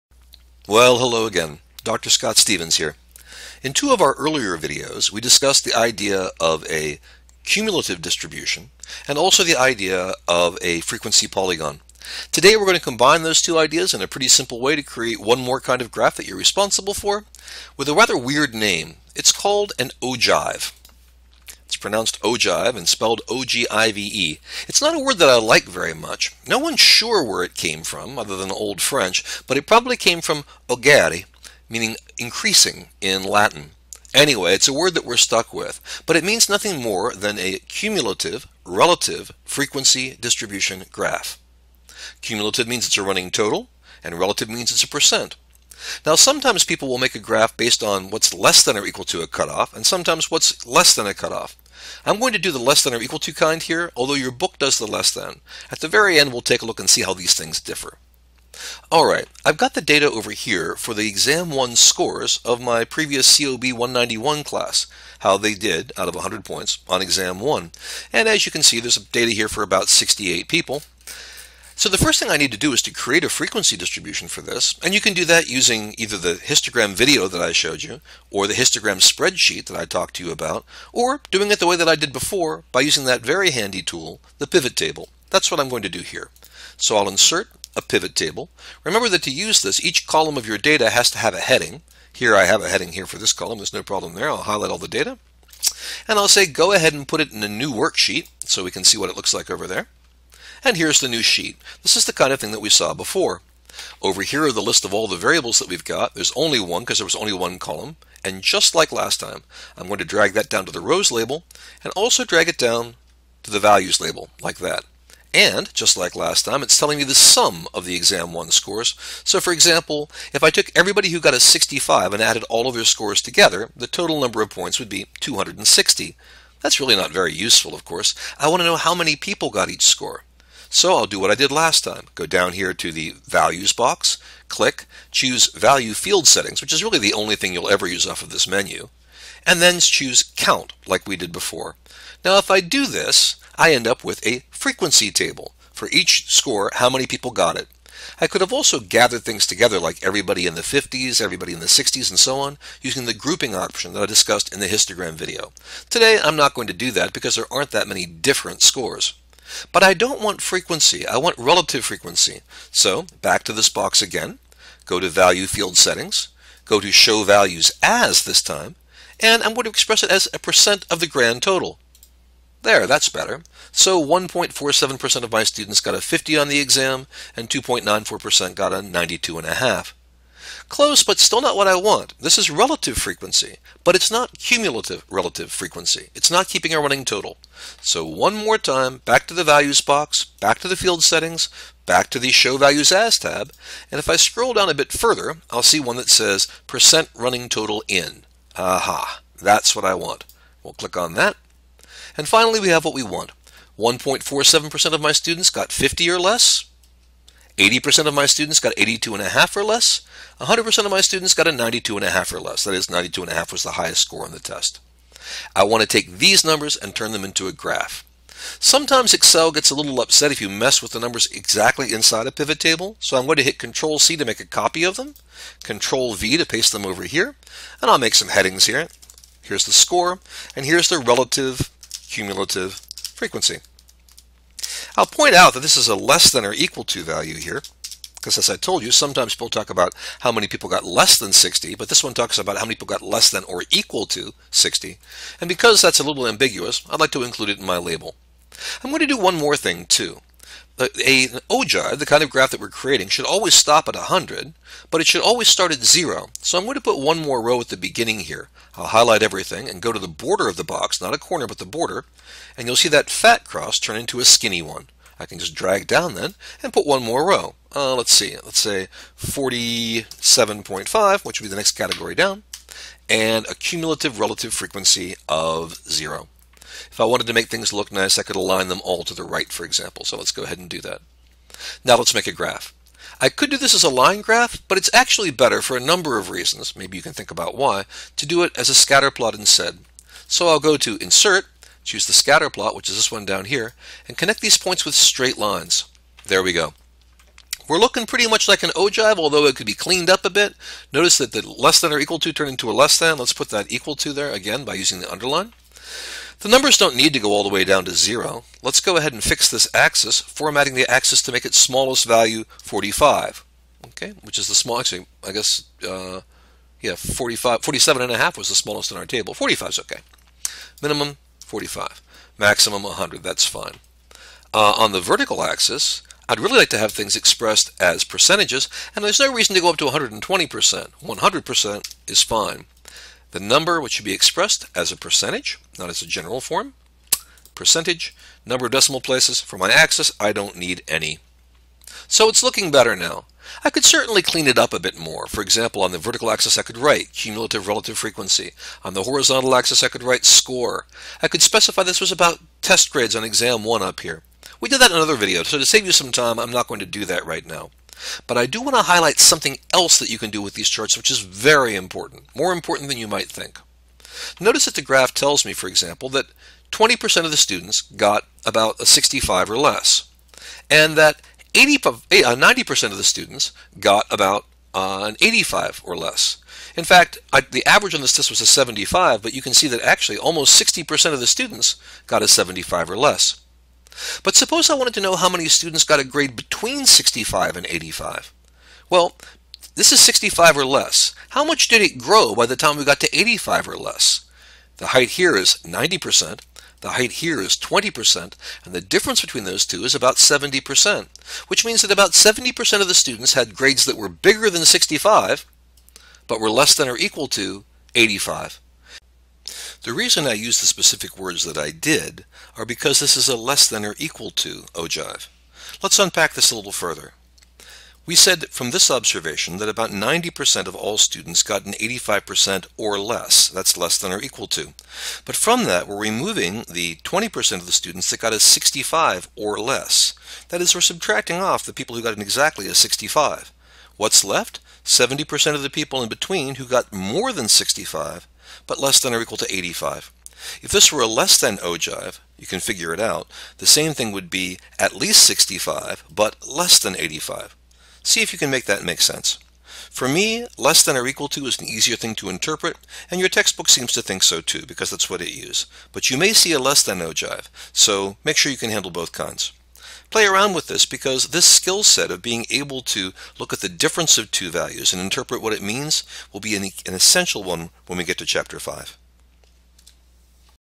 Ogives
Creating an ogive (a cumulative relative frequency polygon) in Excel by using PivotTables, and talking about why ogives are useful.